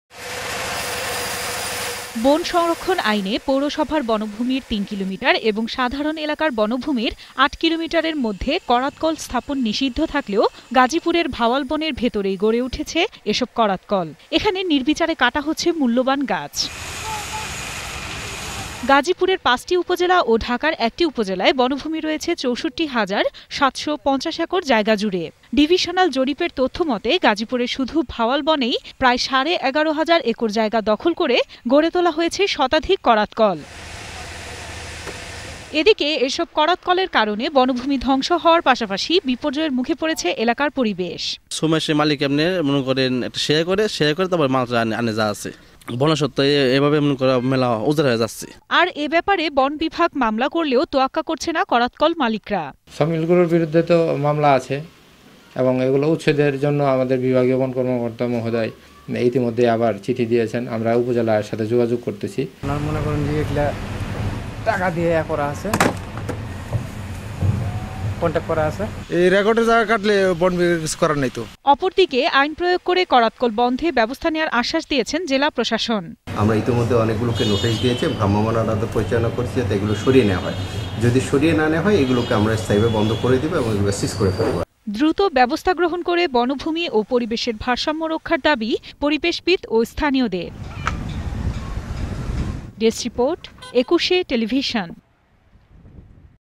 গাজীপুরের বনের ভেতরে শতাধিক অবৈধ করাতকল | ETV News
পরিবেশ আইনের তোয়াক্কা না করে গাজীপুরের ভাওয়াল বনের ভেতরে গড়ে তোলা হয়েছে শতাধিক অবৈধ করাতকল। এসব করাতকলে প্রকাশ্যে কাটা হচ্ছে শাল-গজারীসহ মূল্যবান গাছ। প্রশাসনিক নজরদারি না থাকায় বনাঞ্চল ধ্বংসের পাশাপাশি ঝুঁকিতে রয়েছে এলাকার পরিবেশ। এদিকে বন সুরক্ষায় আইন প্রয়োগের আশ্বাস দিলেও এখন পর্যন্ত কোনো পদক্ষেপ নেয়নি জেলা প্রশাসন।
বন সংরক্ষন আইনে পৌরসভার বনভূমির তিন কিলোমিটার এবং সাধারণ এলাকার বনভূমির ৮ কিলোমিটারের মধ্যে করাতকল স্থাপন নিষিদ্ধ থাকলেও, গাজীপুরের ভাওয়াল বনের ভেতরেই গড়ে উঠেছে এসব করাতকল। এখানে নির্বিচারে কাটা হচ্ছে মুল্যবান গাছ।
গাজীপুরের ৫টি উপজেলা ও ঢাকার ১টি উপজেলায় বনভুমি রয়েছে ৬৪ হাজার ৭শ ৫০ একর জায়গাজুড়ে। ডিভিশনাল জরিপের তথ্যমতে গাজীপুরের শুধু ভাওয়াল বনেই প্রায় সাড়ে ১১ হাজার একর জায়গা দখল করে গড়ে তোলা হয়েছে শতাধিক করাতকল।
এদিকে এসব করাতকলের কারনে বনভূমি ধ্বংস হওয়ার পাশাপাশি বিপর্যয়ের মুখে পড়েছে এলাকার পরিবেশ।
আর এ ব্যাপারে বনবিভাগ মামলা করলেও তোয়াক্কা করছেন না করাতকল মালিকরা।
অপরদিকে আইন প্রয়োগ করে করাতকল বন্ধে ব্যাবস্থা নেয়ার আশ্বাস দিয়েছে জেলা প্রশাসন।
দ্রুত ব্যবস্থা গ্রহণ করে বনভূমি ও পরিবেশের ভারসাম্য রক্ষার দাবি পরিবেশবিদ ও স্থানীয়দের।